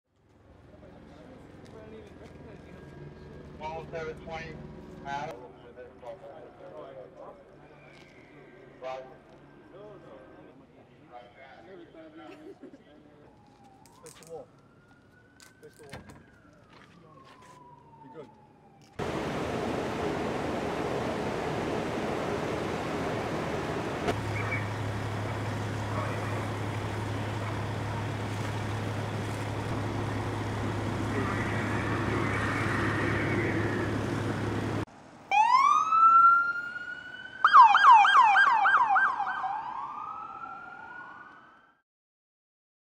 Los Angeles police arrest protesters as US clashes escalate | AFP
Los Angeles police arrest several protesters while firefighters tackle a fire at a looted computer store on Melrose Avenue following violent clashes during a demonstration against the death of George Floyd.